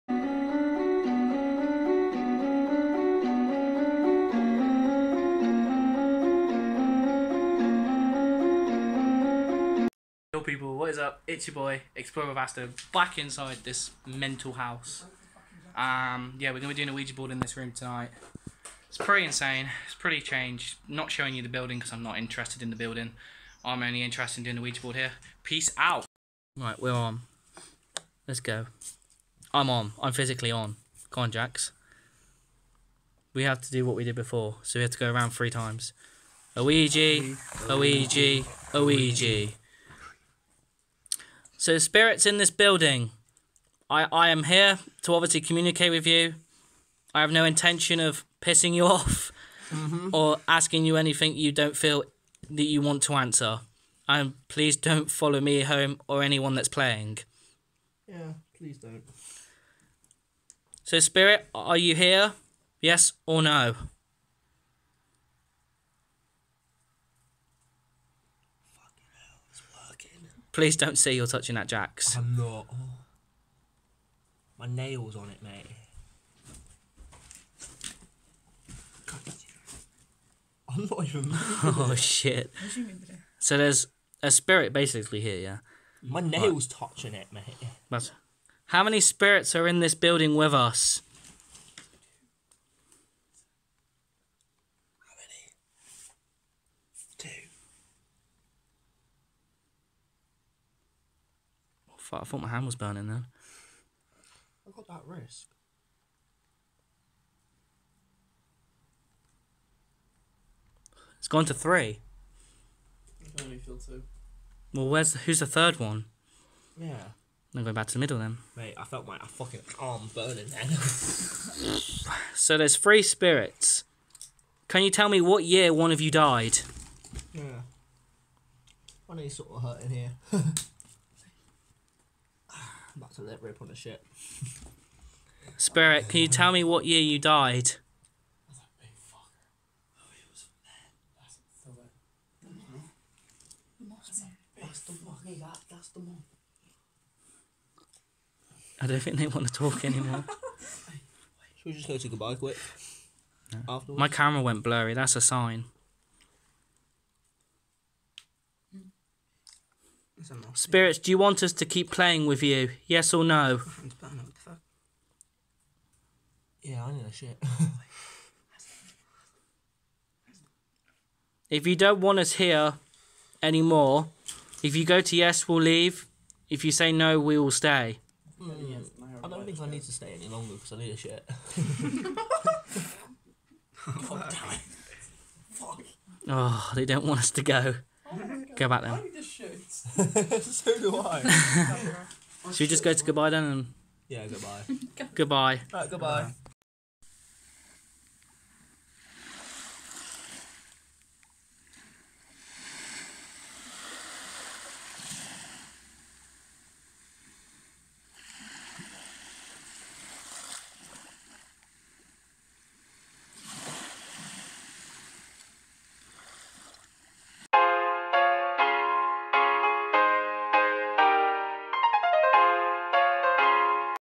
Ouija Board In A Abandoned House | Exploring With Aston
This was very Scary to do this in the abandoned house but it was made at the same time but even before we was filming there was alot going on